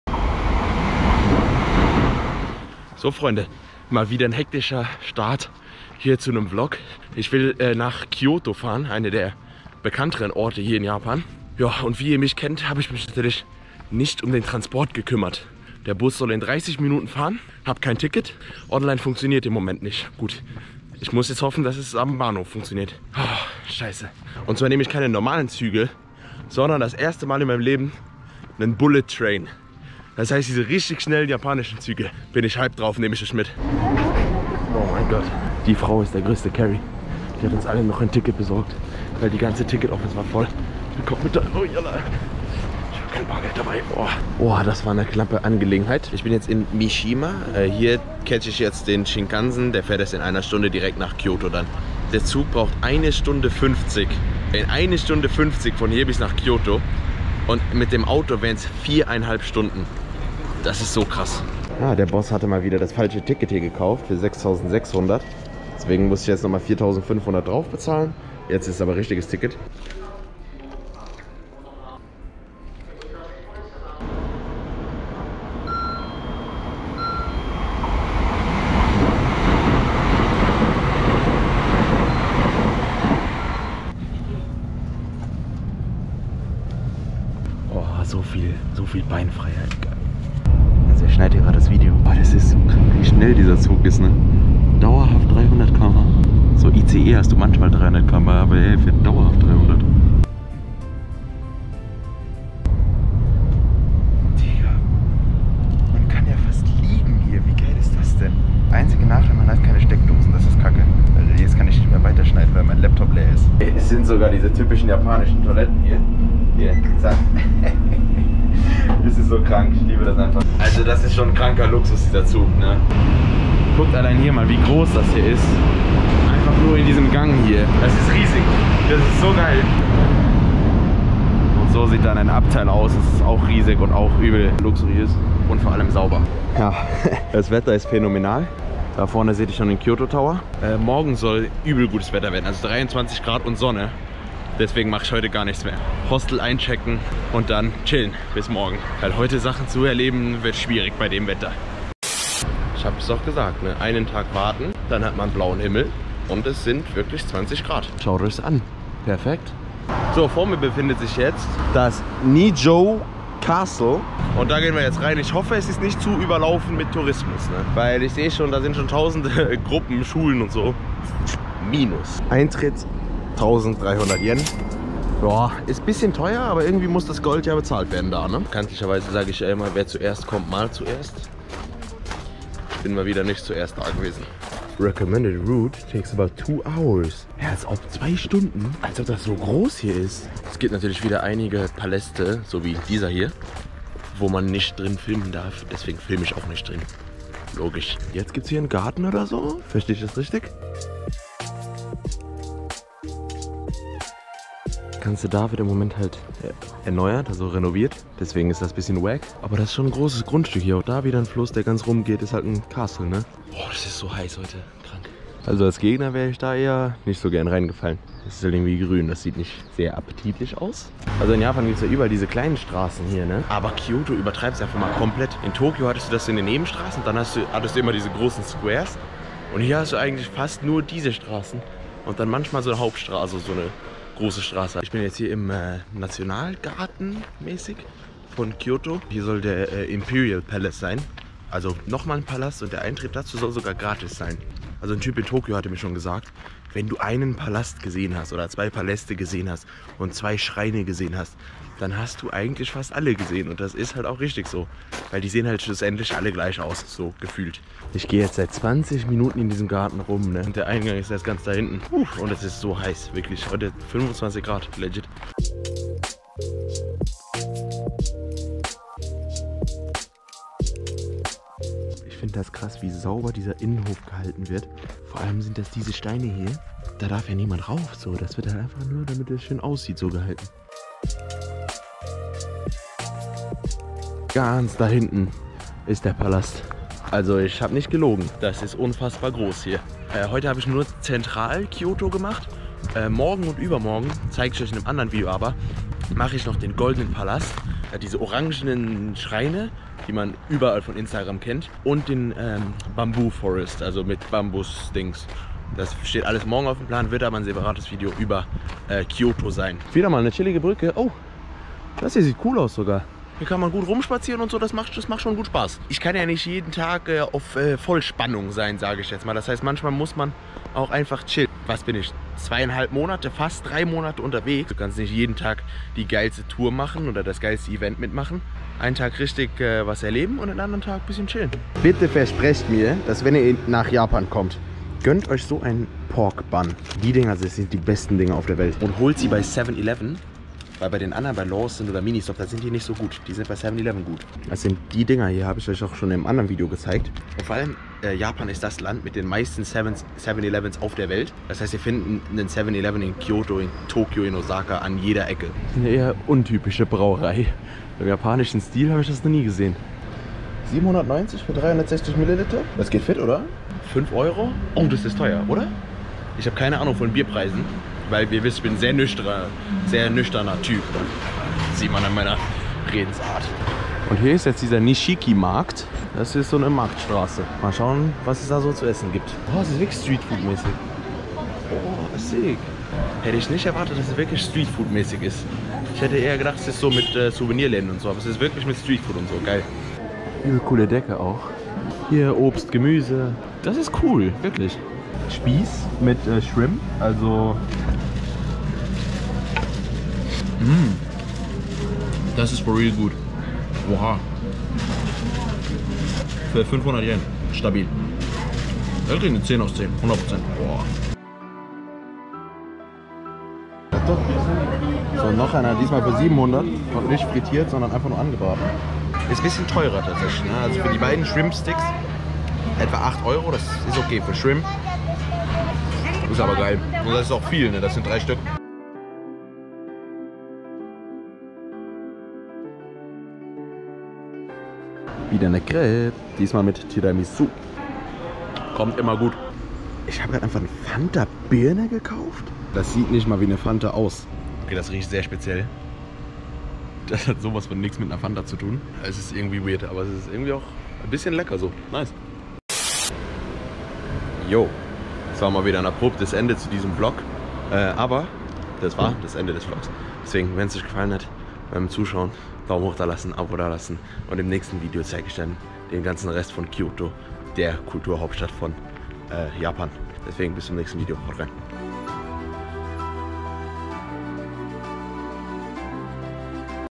Ich TESTE den SCHNELLSTEN Zug Japans!🚅 (300+ km/h)
Neben der Fahrt mit dem Shinkansen Bullet Train nehme ich euch auch mit durch das Zentrum KYOTOs mit zwei Palästen, einem schönen Garten und einem Streetfood Markt.

Checkt auch die Letzten Videos zu meiner Reise ab!

Mein Equipment:
Videos teilweise auch mit dem Handy aufgenommen. (iPhone 12)